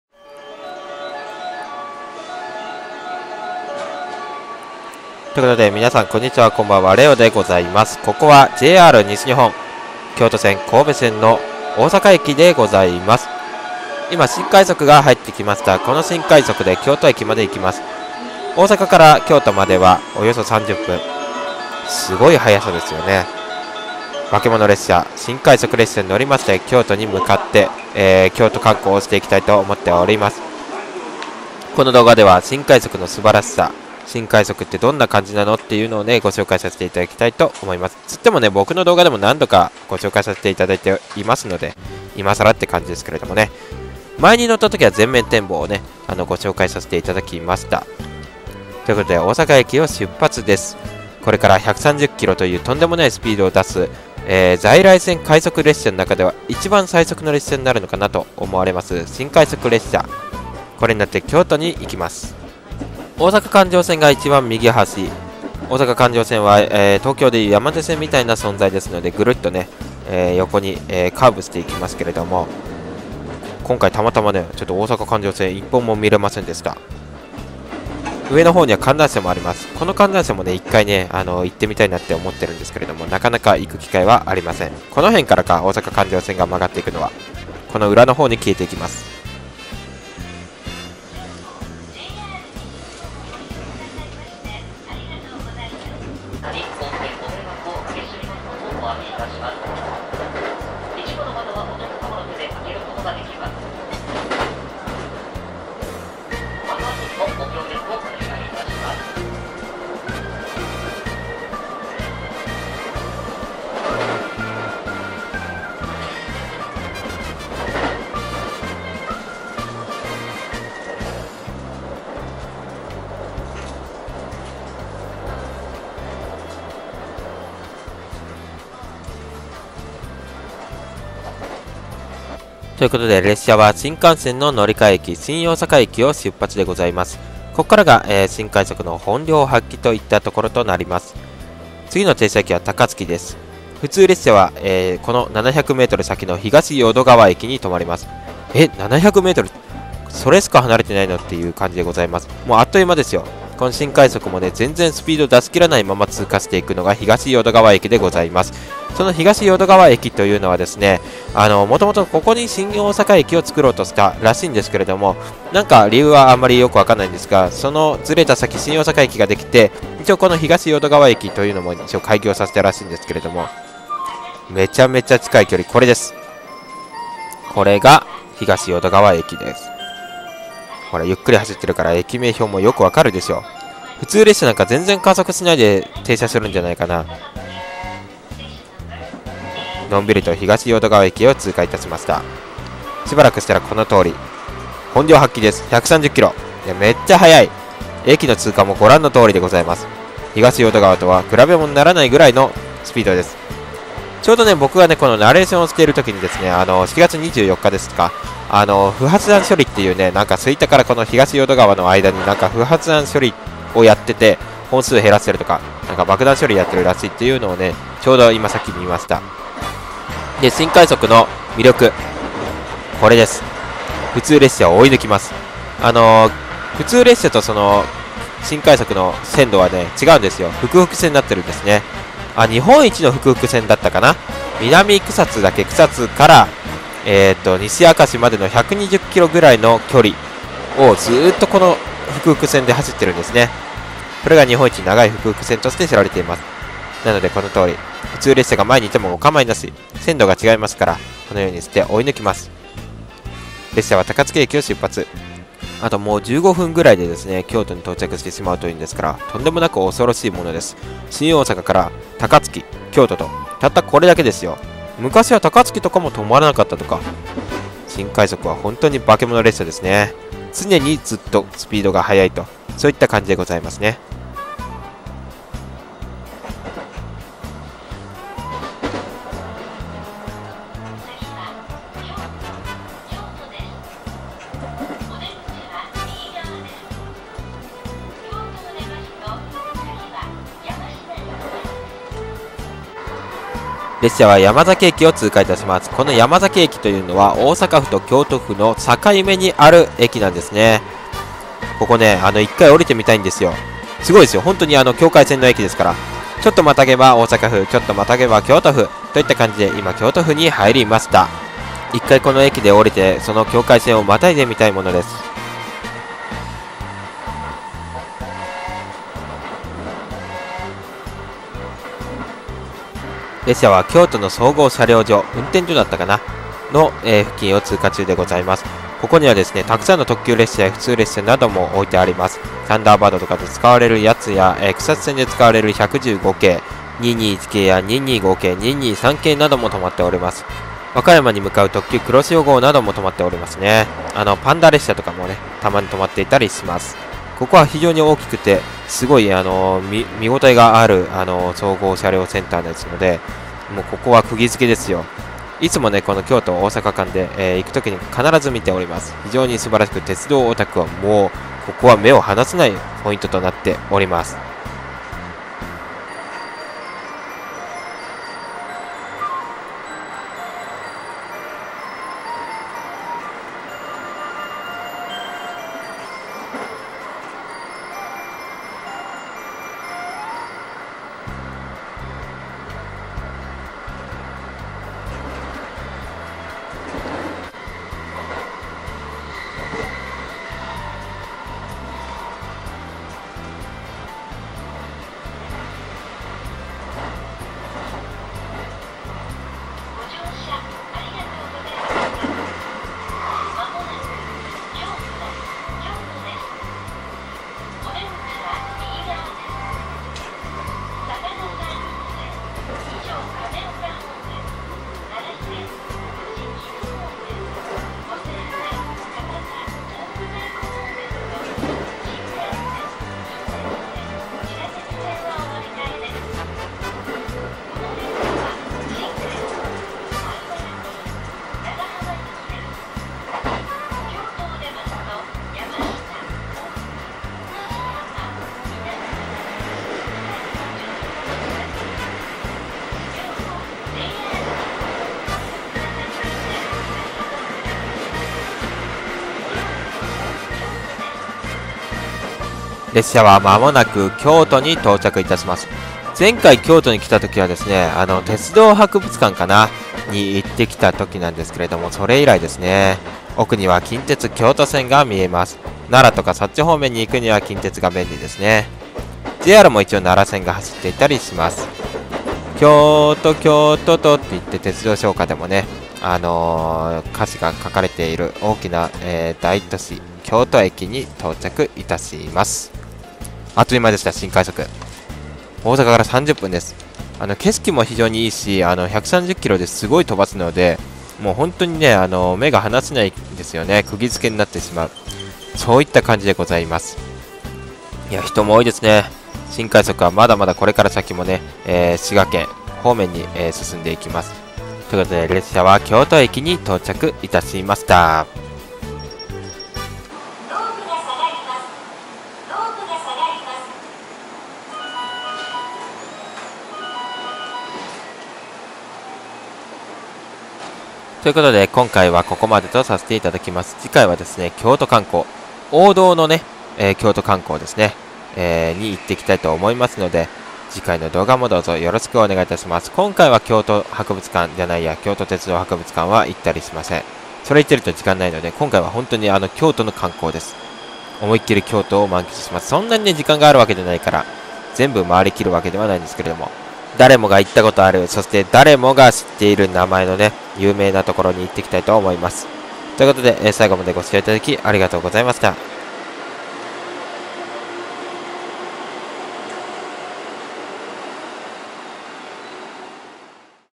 【新快速】JRで1番最強の無課金爆速列車！大阪から京都を新快速で！"大阪〜京都"乗車記
名前 れお